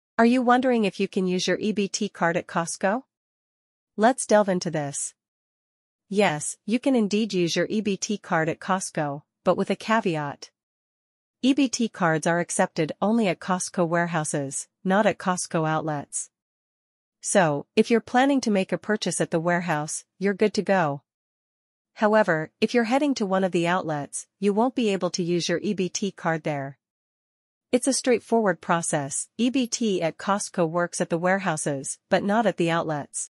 HOW TO USE EBT CARD AT COSTCO 2024!
Learn how to use your EBT card at Costco with our step-by-step guide for 2024. Whether you're shopping for groceries, household items, or electronics, we'll show you how to complete transactions seamlessly using your EBT card at Costco.